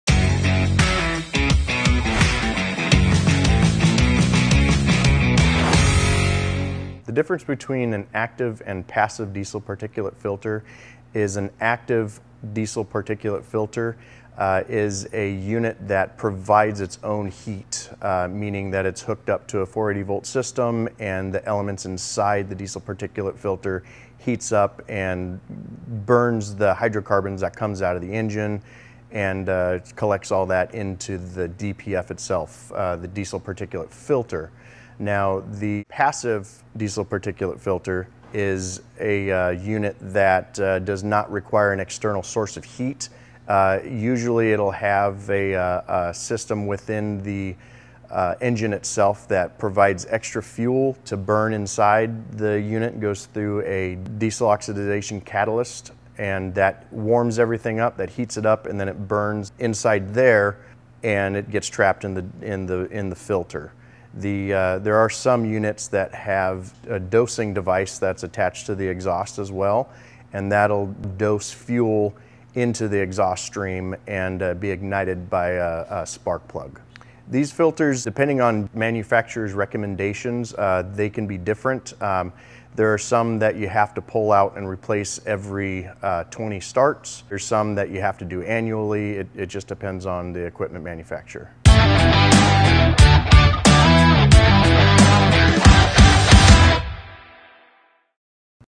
The difference between an active and passive diesel particulate filter
The difference between an active and passive diesel particulate filter is an active diesel particulate filter is a unit that provides its own heat, meaning that it's hooked up to a 480-volt system. The elements inside the diesel particulate filter heat up, burn the hydrocarbons that come out of the engine, and collect all that into the DPF itself, the diesel particulate filter.

The passive diesel particulate filter is a unit that does not require an external source of heat. Usually, it'll have a system within the engine itself that provides extra fuel to burn inside. The unit goes through a diesel oxidization catalyst that warms everything up, that heats it up, then it burns inside there, and it gets trapped in the filter.

There are some units that have a dosing device that's attached to the exhaust as well, and that'll dose fuel into the exhaust stream and be ignited by a sparkplug. Depending on the manufacturer's recommendations, these filters can be different. There are some that you have to pull out and replace every 20 starts. There are some that you have to do annually. It just depends on the equipment manufacturer.